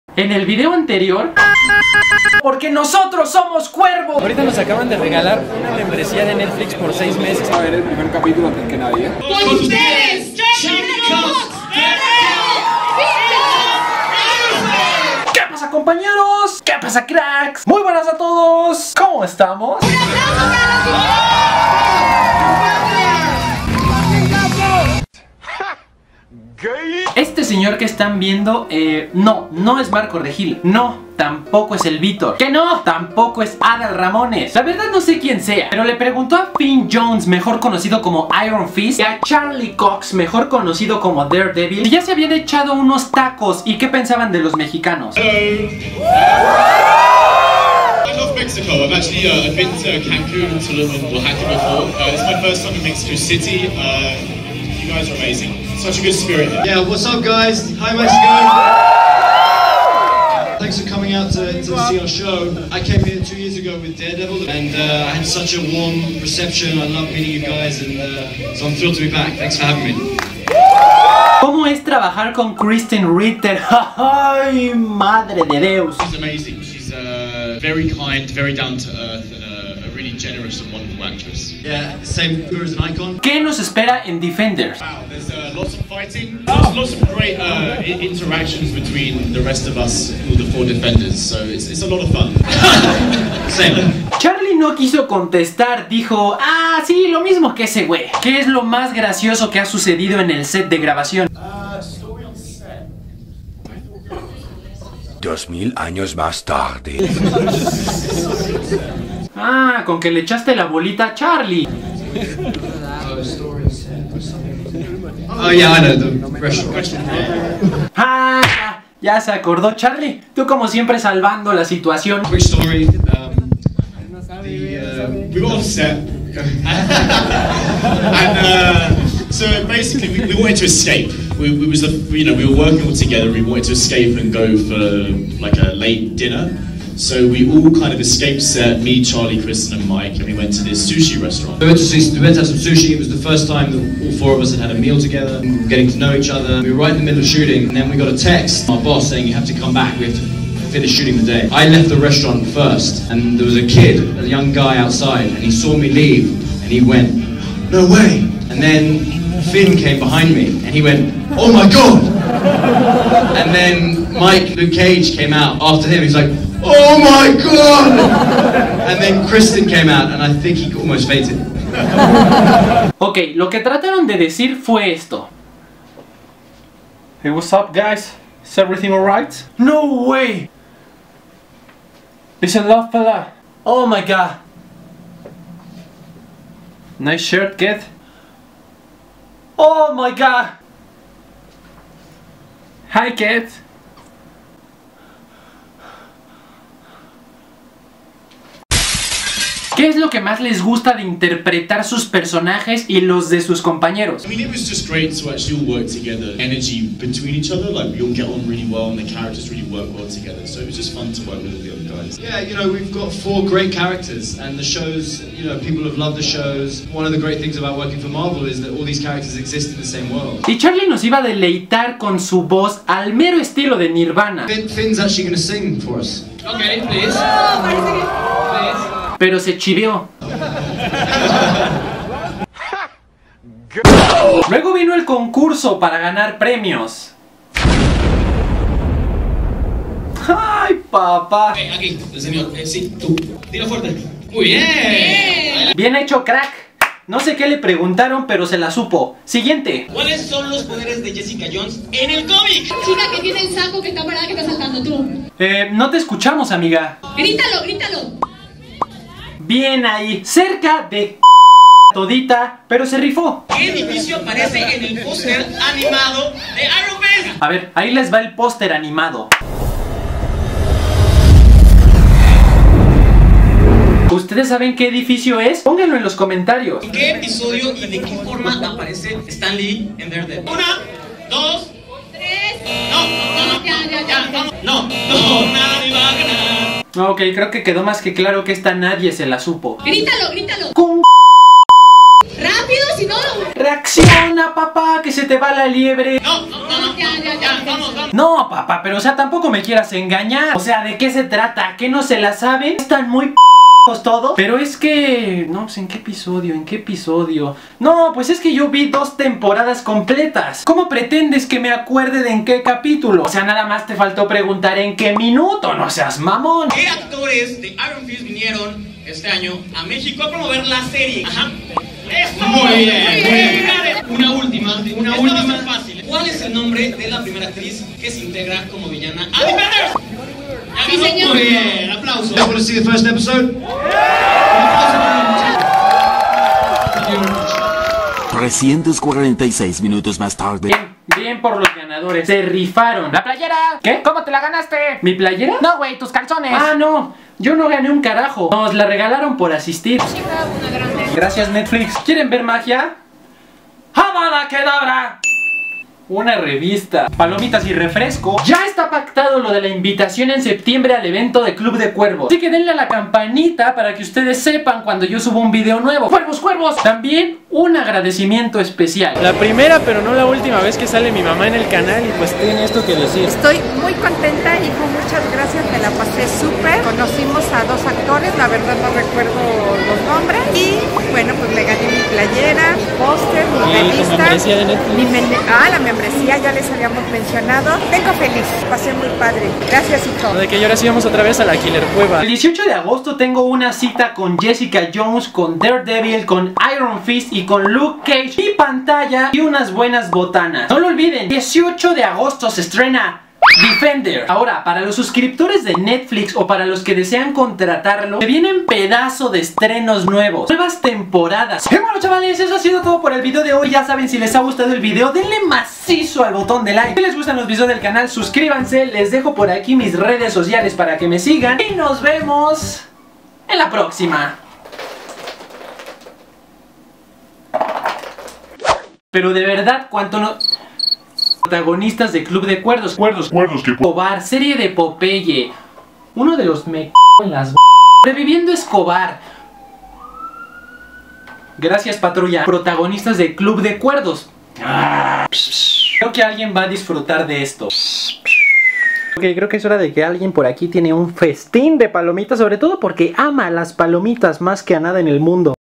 VI el PRIMER CAPITULO DE DEFENDERS | CONOCI a IRON FIST Y DAREDEVIL!
Asistí a un evento de Netflix donde íbamos a conocer a Chava e Isabel Iglesias de Club de Cuervos por la temporada 3 pero no pudieron ir... Pero los que si llegaron fueron los actores de la serie Defenders de Marvel, DareDevil, Iron Fist, Jessica Jones y Luke Cage, vimos el capitulo 1 completo subtitulado en español de la serie completa antes que nadie!

► Sígueme BROU!